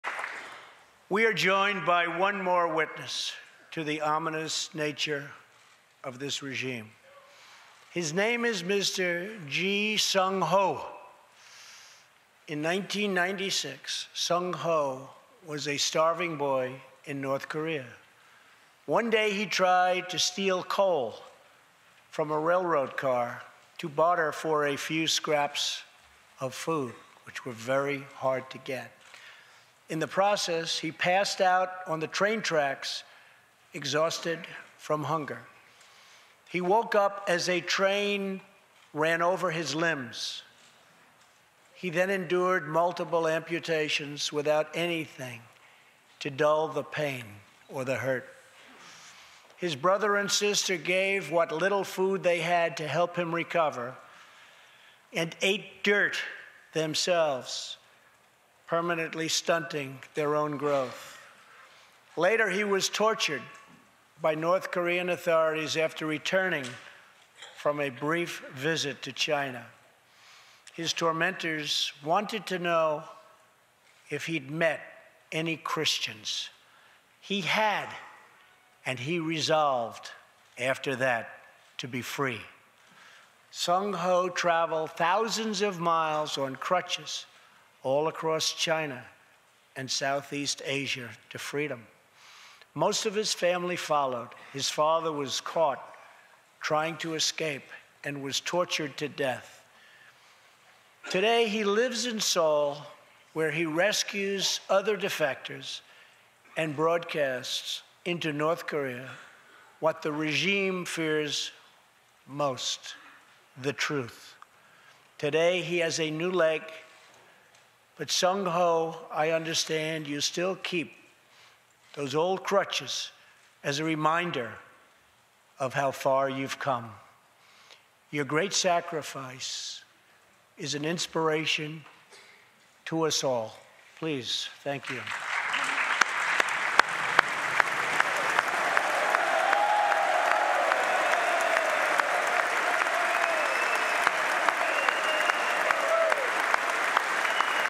Trump calls North Korean defector an inspiration
President Trump praises North Korean defector Ji Seong-Ho during the State of the Union address. Seong-Ho currently lives in Seoul, where he works to help fellow defectors.